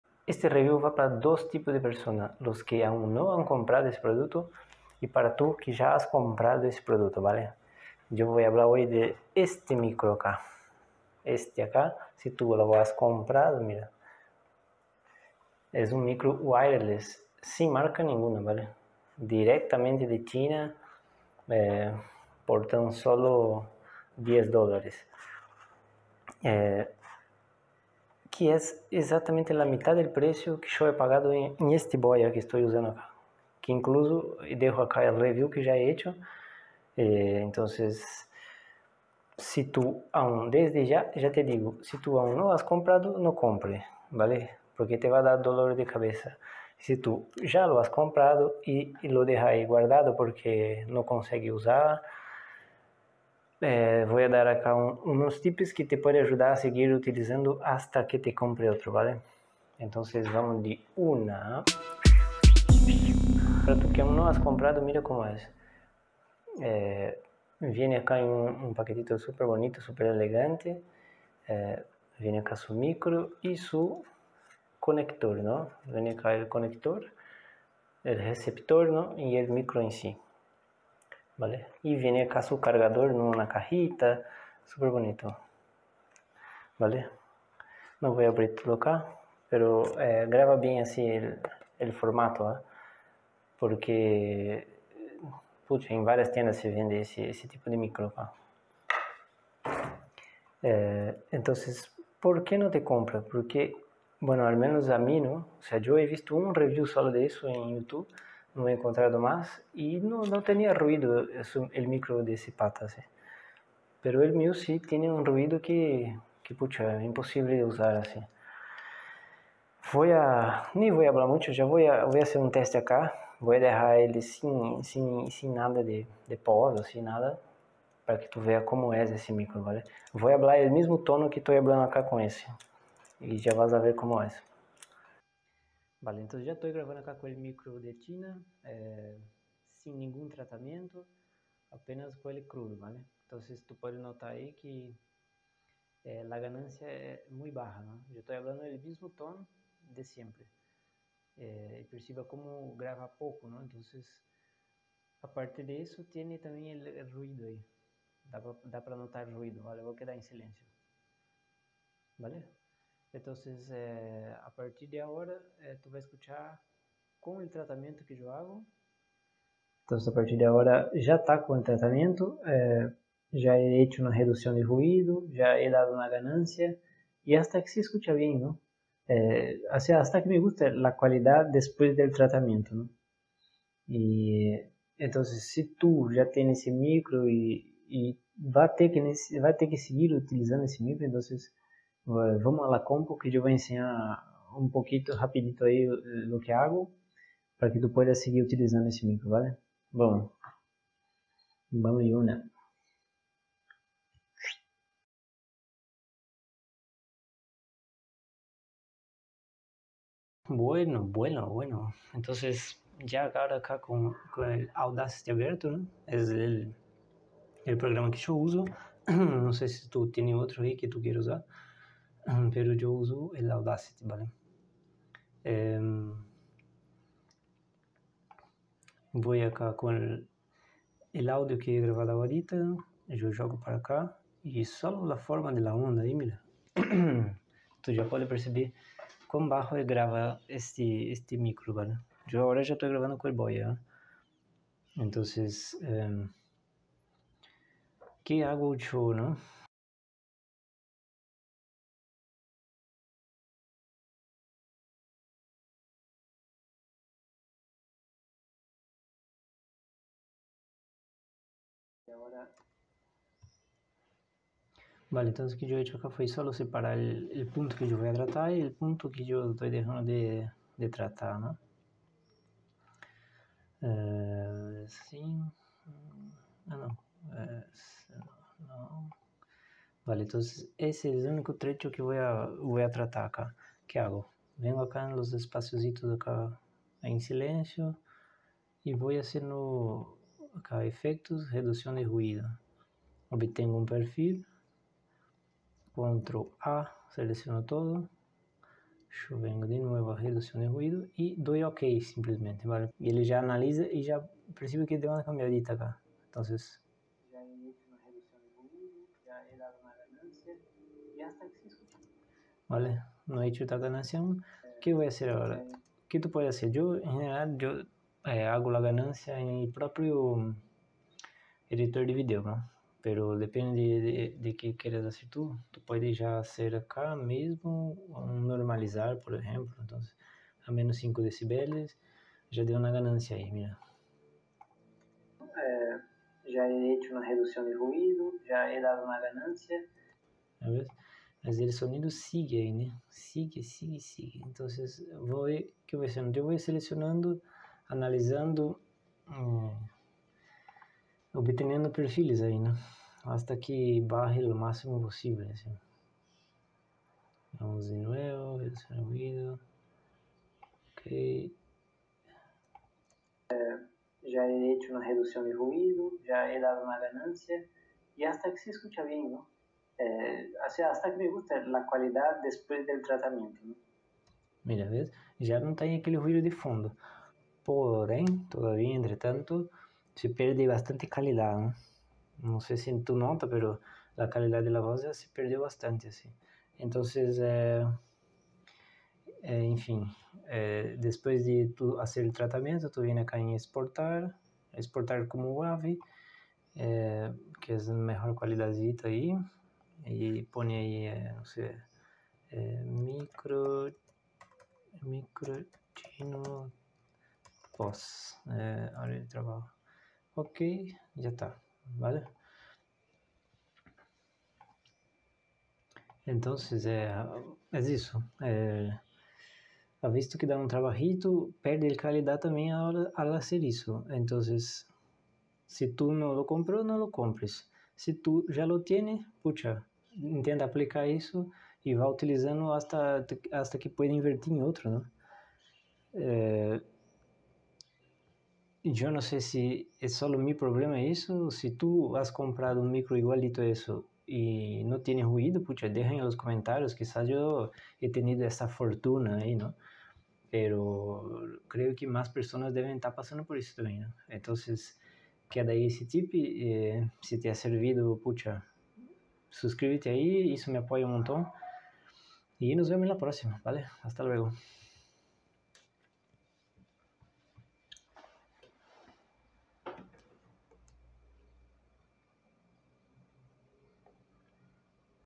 Microfono Inalambrico de China: Vale la pena?? Review
Hablando un poco de ese microfone Wireless made in china, comprado en la tienda Shopee.
Aca enseño como tratar el audio caso haya comprado ese micro y te vino de regalo el tal del ruido de fondo...